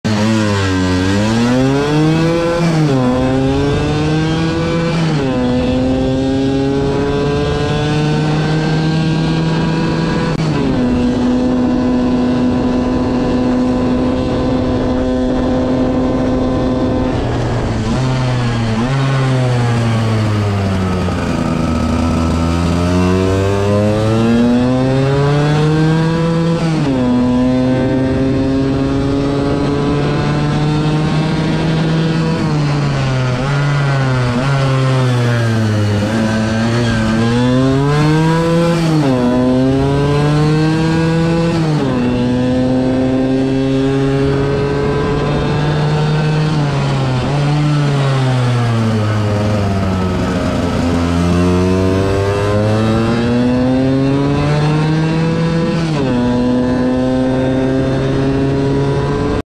Cronoscalata PFP - Vespa PK50 XL Olympia 102cc lamellare
Tempo di 0:59.

PK50 XL (Bianca)
- Olympia 102cc in ghisa 3 travasi con pistone DR, rifasato, spessorato, scarico ampliato, testa fresata.
- Albero originale
- Primaria 16/69
- PWK 24 (Replica)
- Lamellare Polini
- Marmitta Proma
- Accensione VMC